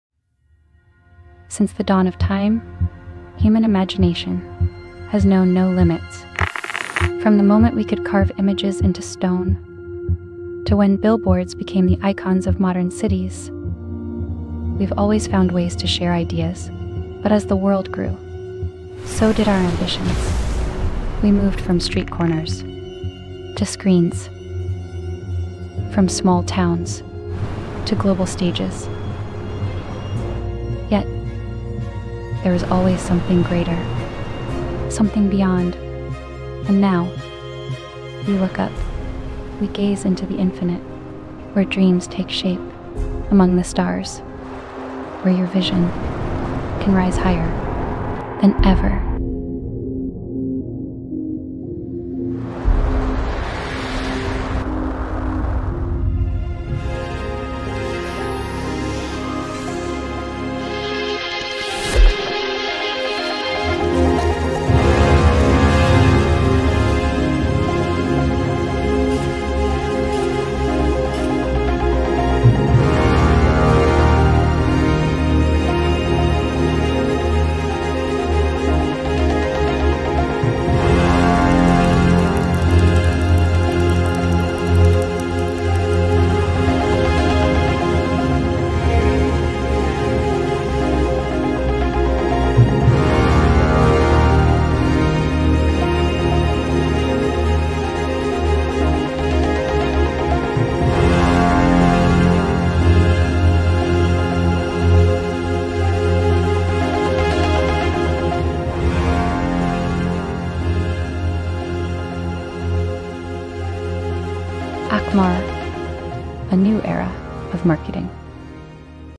Space Advertising | Powered by AQMAAR
AQMAAR is launching a groundbreaking "space-based advertising" mission using advanced satellite swarms and cutting-edge laser technology to project visible advertisements from orbit over major cities around the world.

Our competitive edge lies in two key innovations:

Swarm Control – Our state of art algorithms coordinate fleets of satellites with unmatched precision, enabling real-time optimization of positioning and timing to ensure maximum visual impact.

Laser Technology – We’ve developed highly efficient, space-grade, multi-color laser systems that are visible from Earth—shining as brightly as five stars combined—and fully customizable for dynamic, city-specific campaigns.

Each city in the world will receive a dedicated 3-minute ad slot every night between 8:00 PM and 10:00 PM, with content tailored to the geographic, cultural, and economic profile of its audience—ensuring relevance, engagement, and powerful brand visibility.

Join AQMAAR as we light up the sky and redefine the future of advertising—one city at a time.